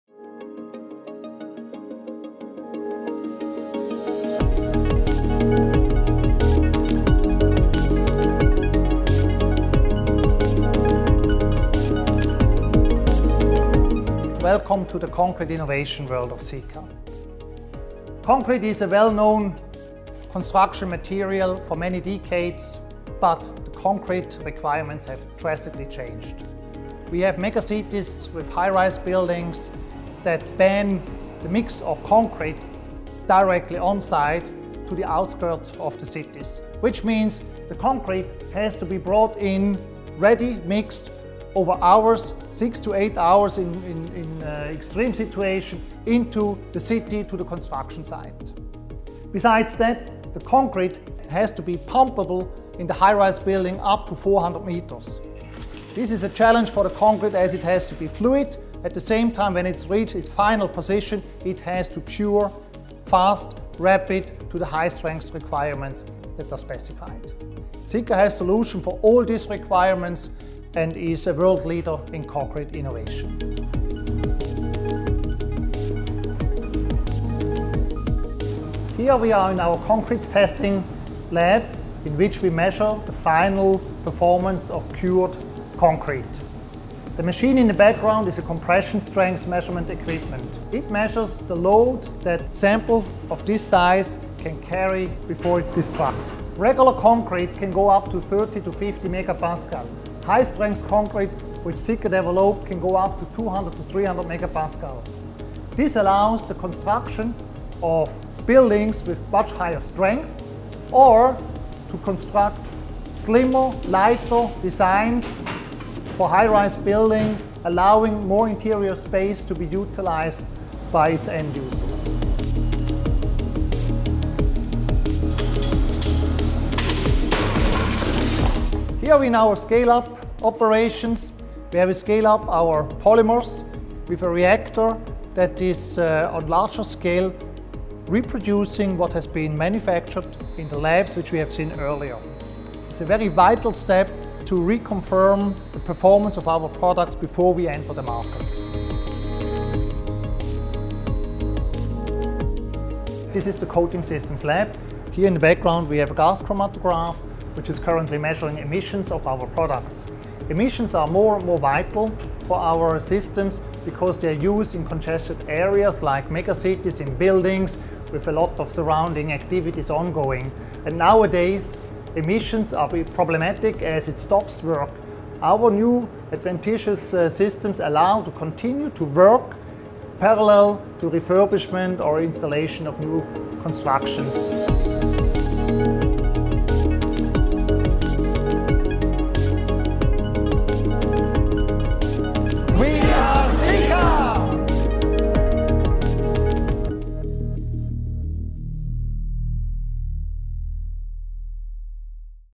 Sika R&D - Part 2 - Megacities
Sika's Research and Development team talks about innovations within Sika - Part 2